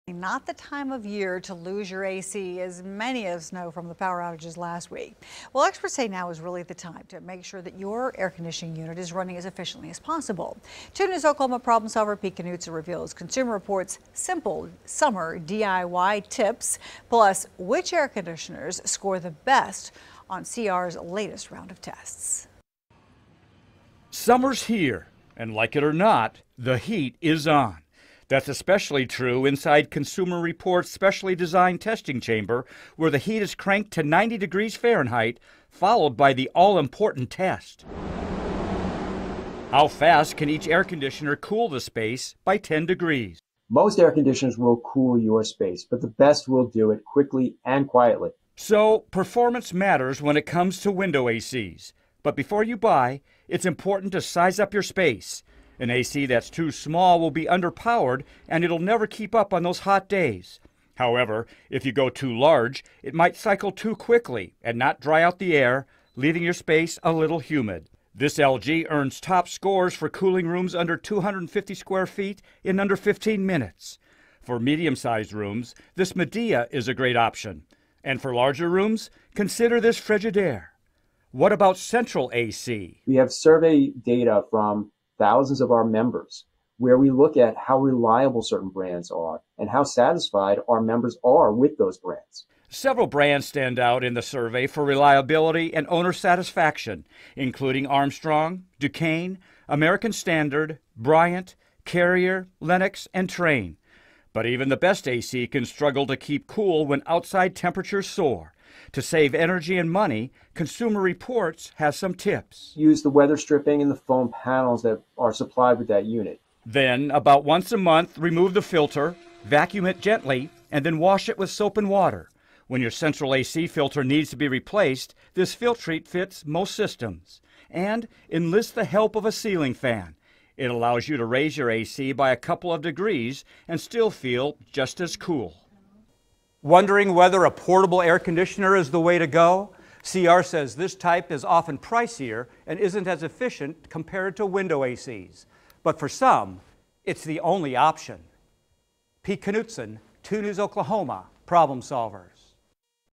Consumer Reports: Air Conditioner Tests
Weather experts are predicting another scorcher of a summer for many parts of North America. With many of us still reeling from winter heating costs, now is the time to make sure your A/C is running as efficiently as possible. Consumer Reports reveals some simple summer DIY tips, plus which A/Cs score the best from its latest round of tests.

Performance matters when it comes to window A/Cs. But before you buy, it’s important to size up your space. An A/C that’s too small will be underpowered and will never keep up on those hot days.

2 News' Pete Knutson reports.